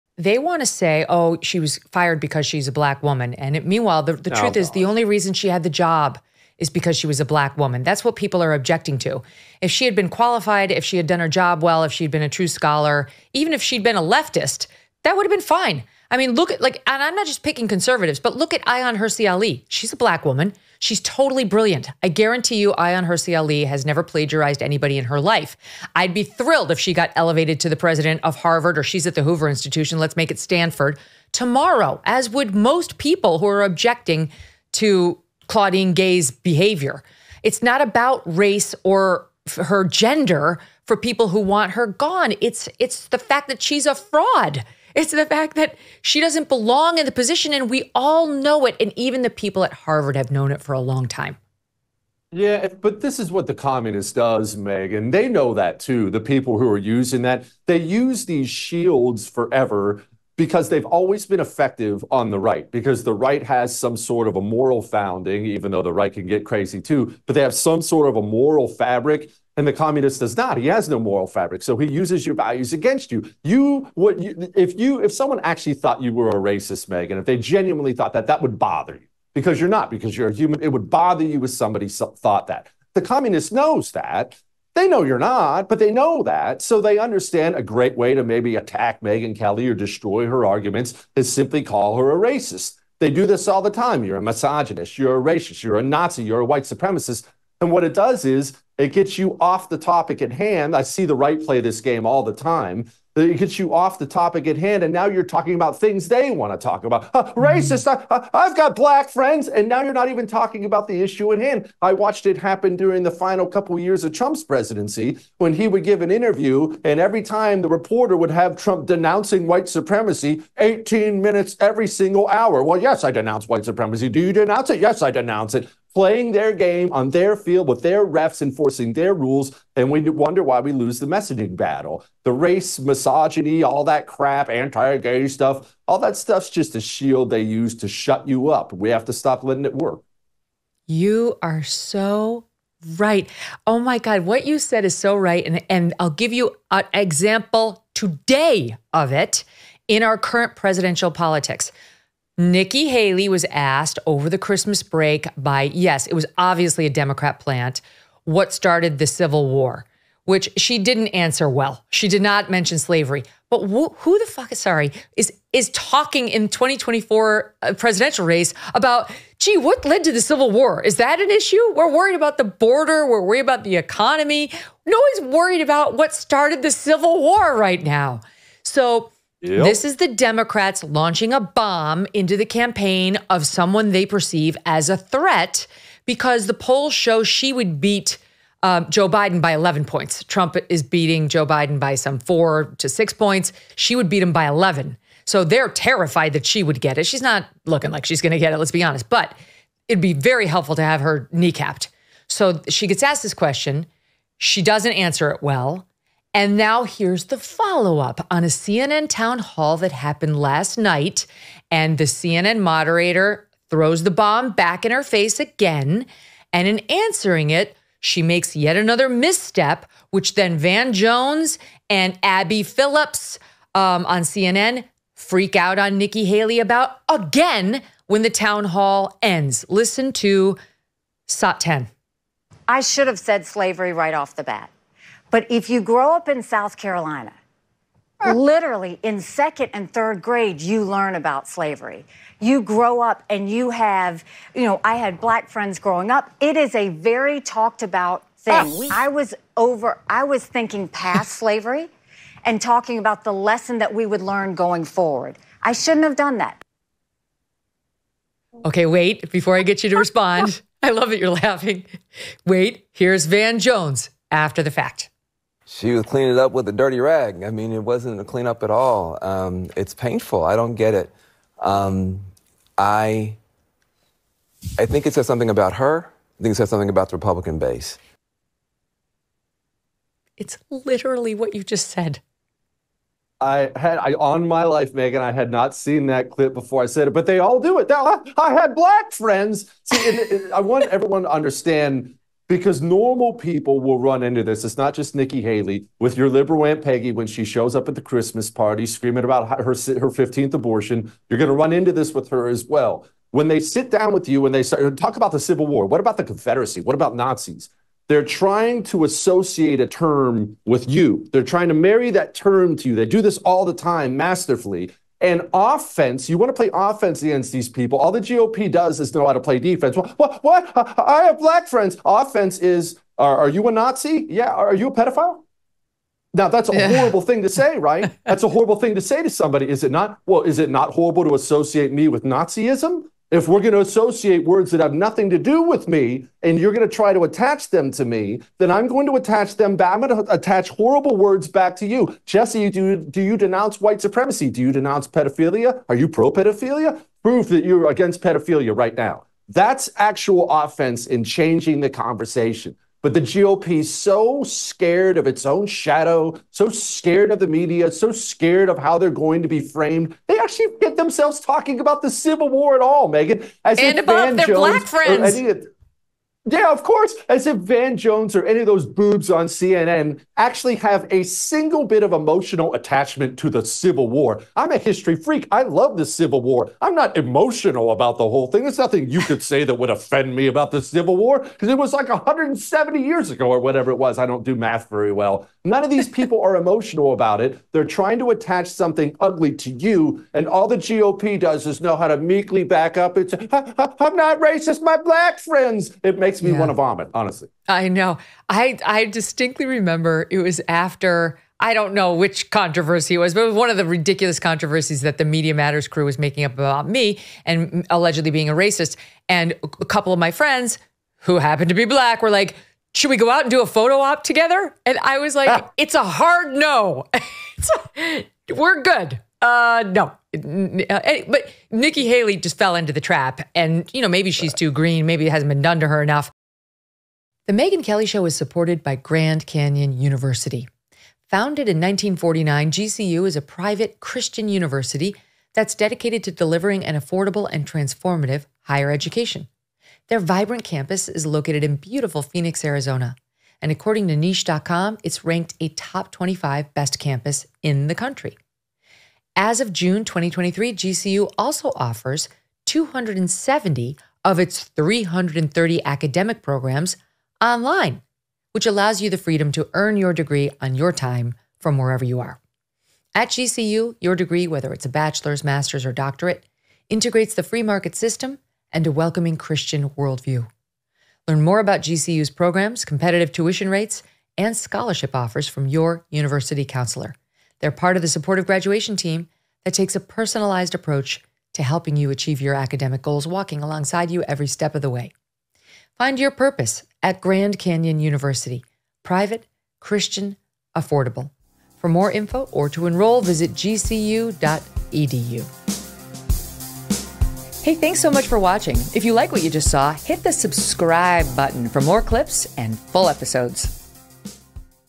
Nikki Haley Falls Into Democrats' Trap with Civil War and "Black Friends" Comments, with Jesse Kelly
Megyn Kelly is joined by Jesse Kelly, host of TheFirst TV’s “I’m Right,” to discuss Nikki Haley falling into the Democrats’ trap with recent Civil War comments, her latest attempt at clean up by invoking “black friends," and more.